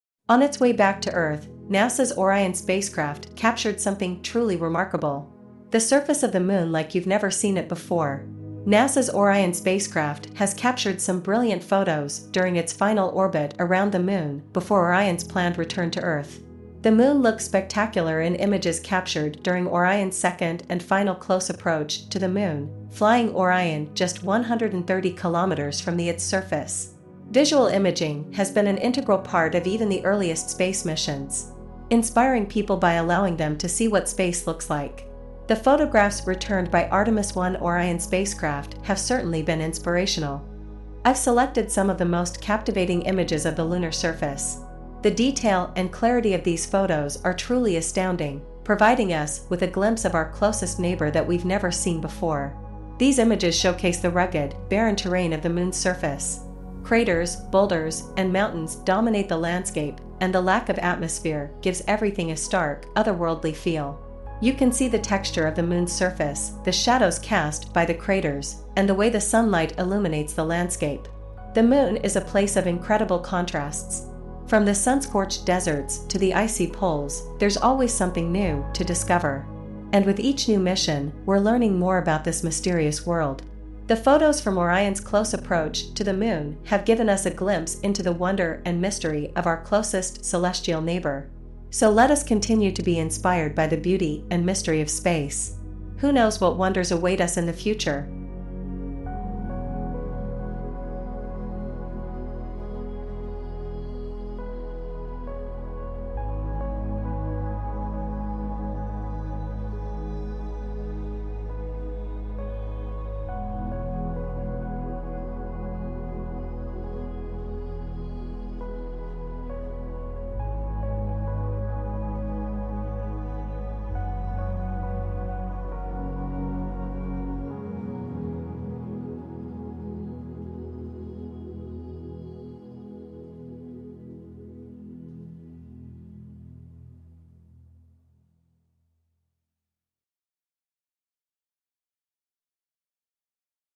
Artemis 1 Aftermath: Stunning Photos of the Moon's Surface
On its way back to Earth, NASA's Artemis 1 Orion spacecraft captured something truly remarkable. The surface of the Moon like you've never seen it before.

NASA's Orion spacecraft has captured some brilliant photos during its final orbit around the moon before Orion's planned return to Earth.

The moon looks spectacular in images captured during Orion's second and final close approach to the Moon, flying Orion just 130 km from the its surface.

You can see the texture of the Moon's surface, the shadows cast by the craters, and the way the sunlight illuminates the landscape.

The Moon is a place of incredible contrasts. From the sun-scorched deserts to the icy poles, there's always something new to discover. And with each new mission, we're learning more about this mysterious world.